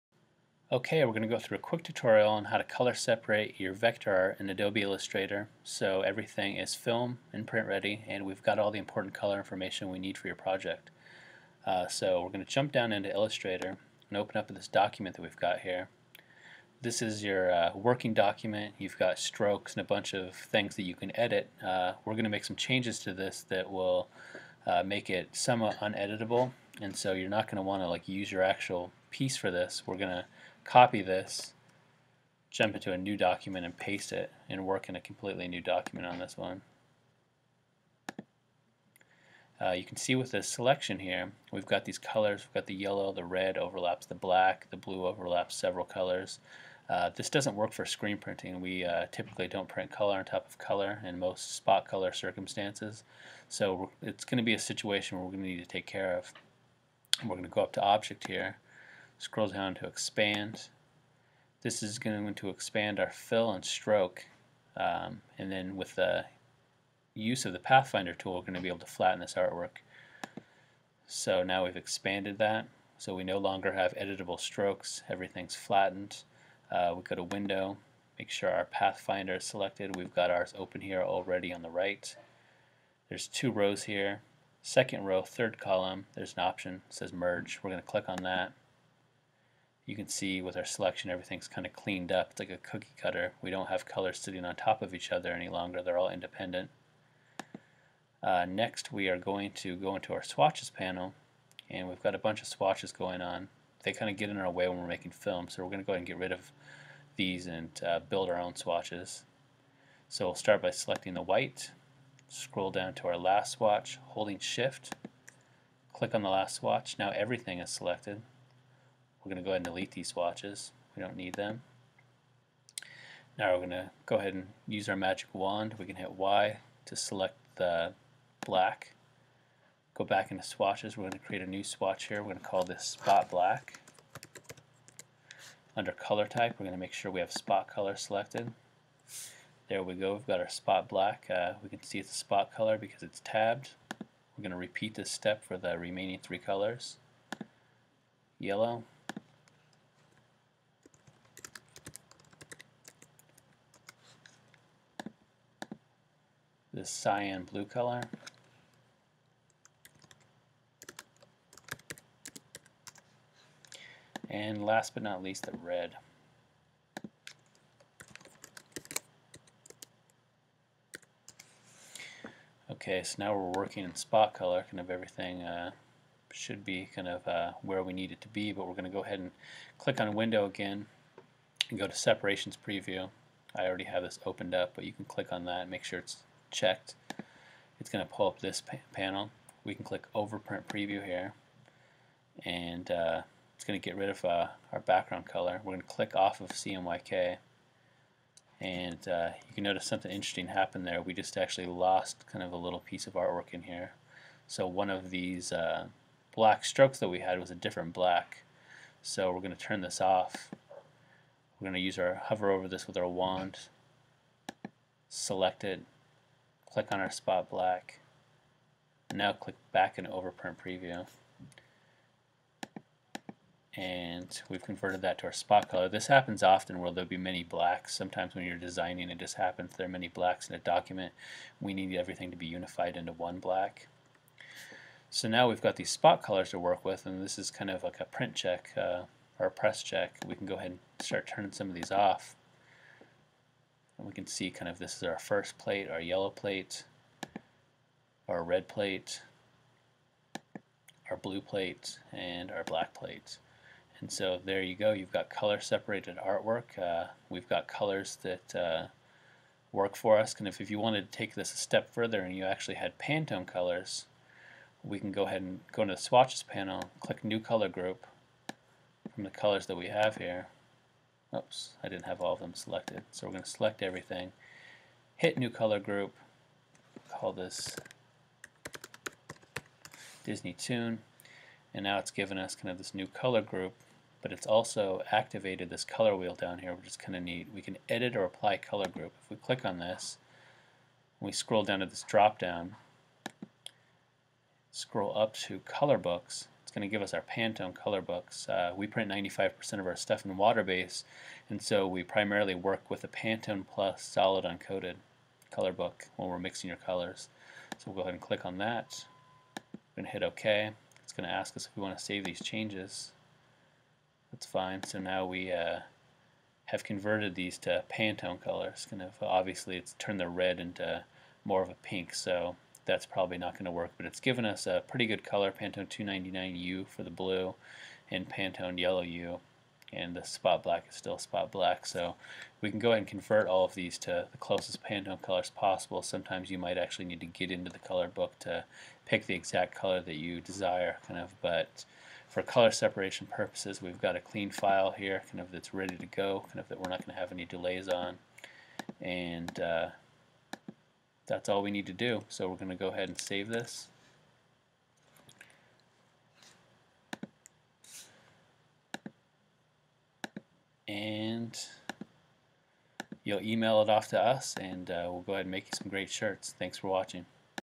Stop Paying for Color Separations – Do It Yourself in Illustrator!
Want to save money on your screen printing projects and take full creative control of your art?
In this quick Adobe Illustrator tutorial, you’ll learn how to separate colors in vector art to make print-ready films for screen printing — no paid service required.

🎯 What You’ll Learn:
 • Step-by-step color separation in Adobe Illustrator
 • How to prep artwork for screen print films
 • Pro tips to avoid common beginner mistakes

💡 Whether you’re a graphic designer, print shop owner, or DIY merch brand, this technique will help you streamline your process and keep more cash in your pocket.

🛠 Tools Used: Adobe Illustrator
📸 Watch in HD for best results!